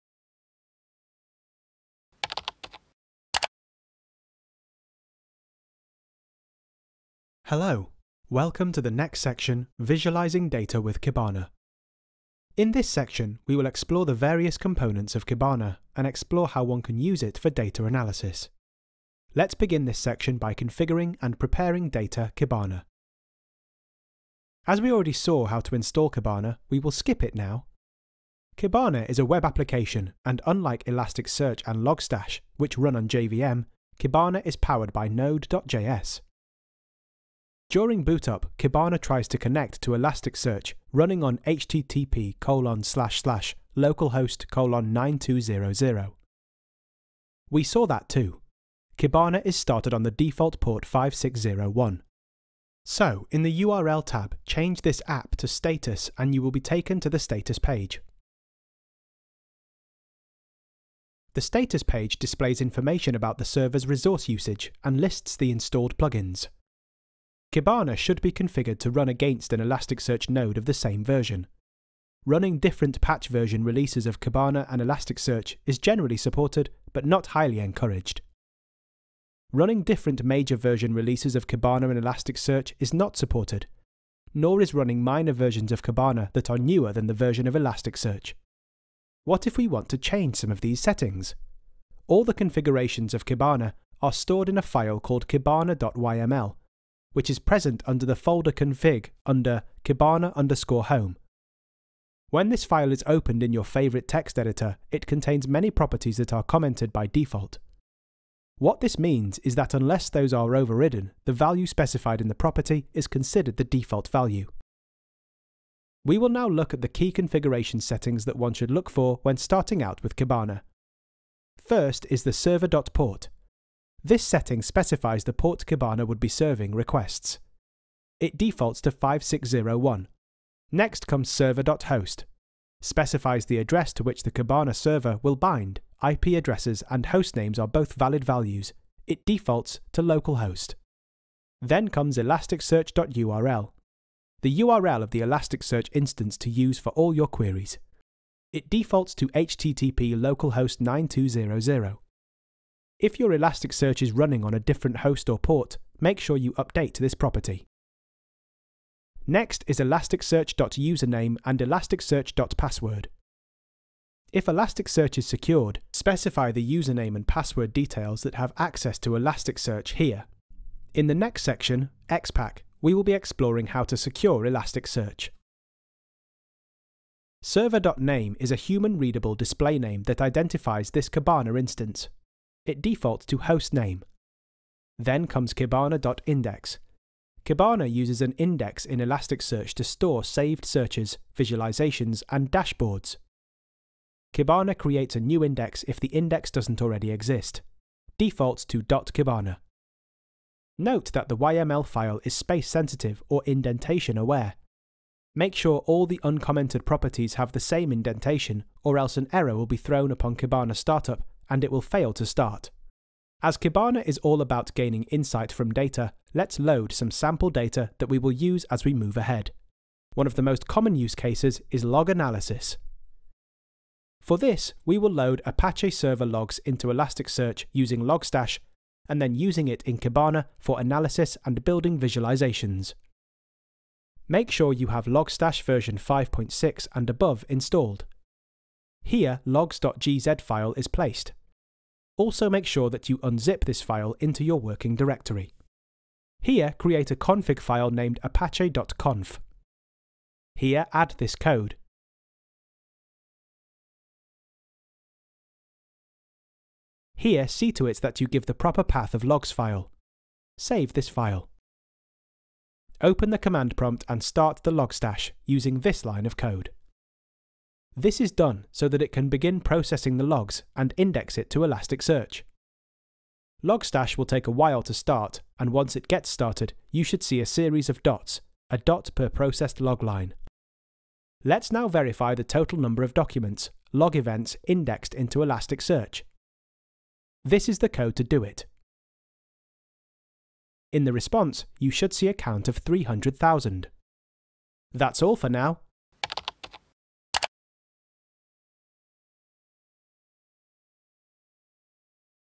Learning Elastic Stack 6.0: Getting Started with Kibana | packtpub.com
This video tutorial has been taken from Learning Elastic Stack 6.0. You can learn more and buy the full video course